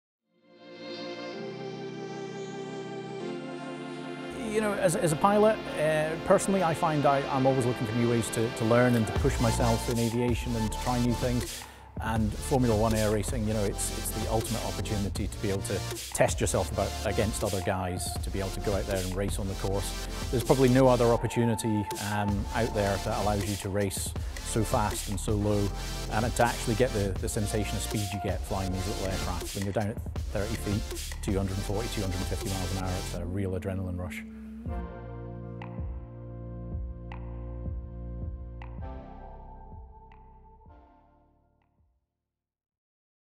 Des Hart: Why I Love Air Race 1
AirRace1 World Cup 2017 present by Chang takes place at U-Tapao, Thailand from 17 to 19 November 2017.

Get all the latest information about Air Race 1 and the World Cup via our other channels;